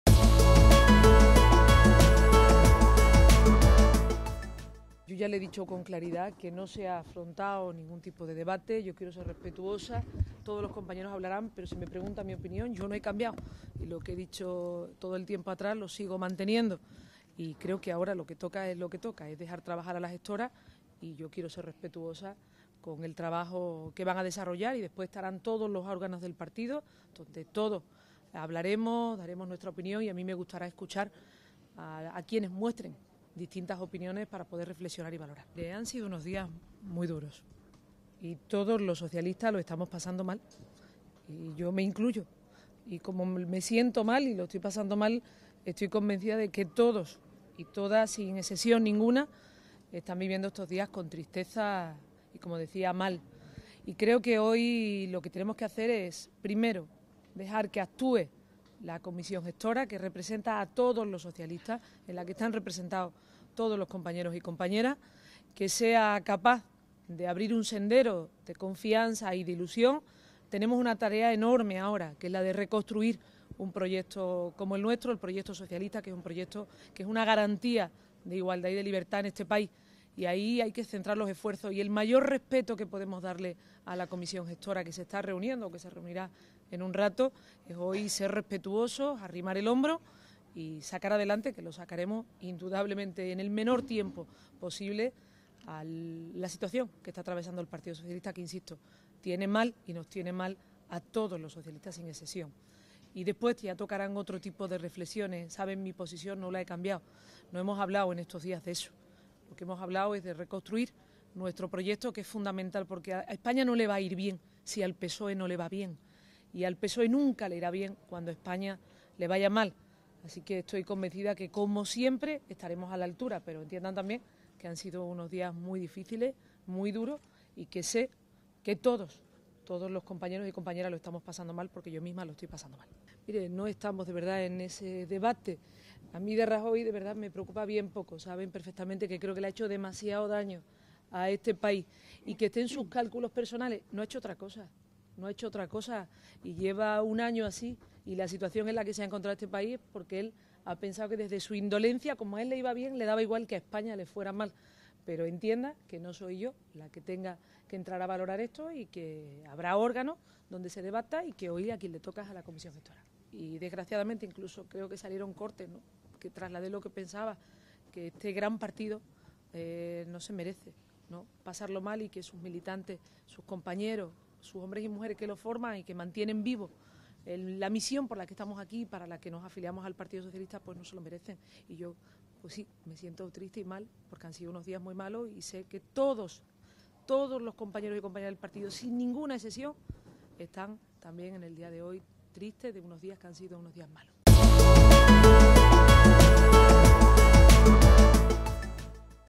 Susana Díaz: "Lo que toca es dejar trabajar a la gestora"
La presidenta de la Junta de Andalucía ha reconocido que "todos los socialistas lo están pasando mal" y ha pedido "dejar trabajar a la gestora". "Rajoy me preocupa muy poco", ha sentenciado.